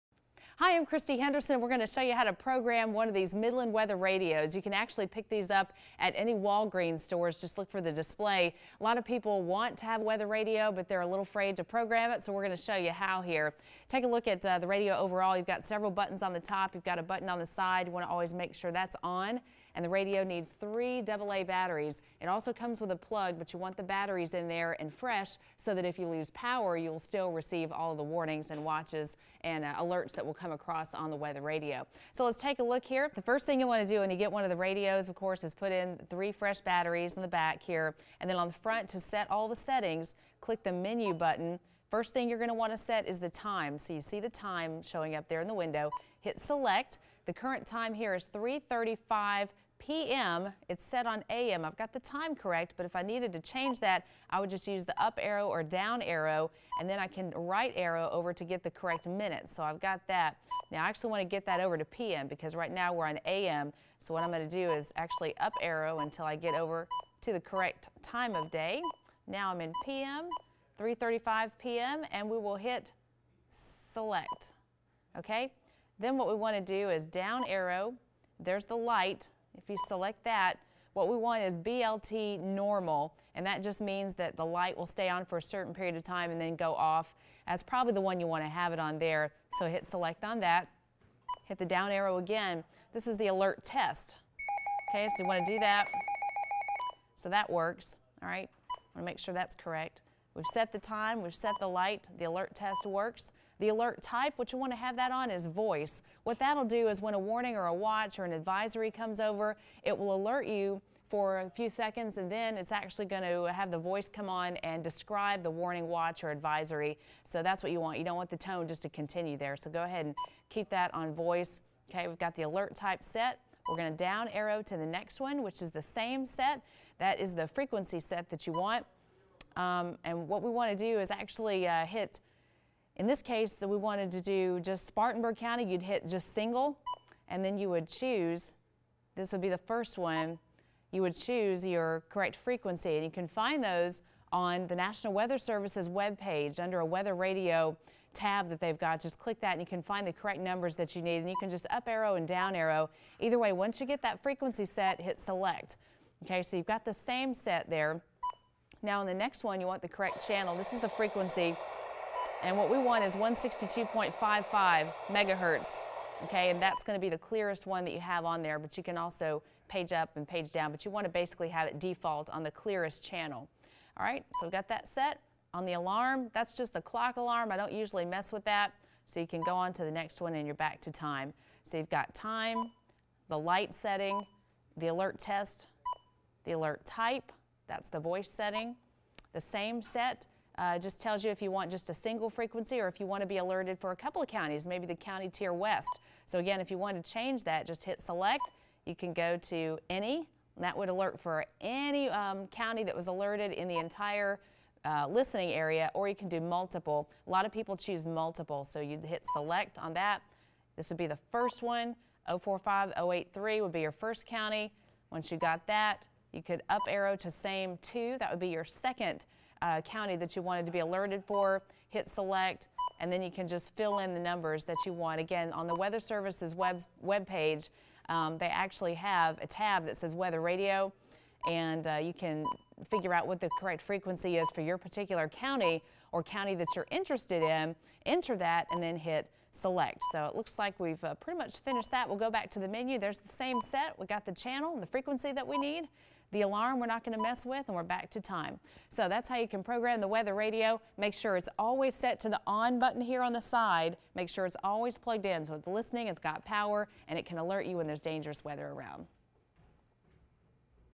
How to Program A Weather Radio
Learn how to program a weather radio from 24/7 weather's Christy Henderson.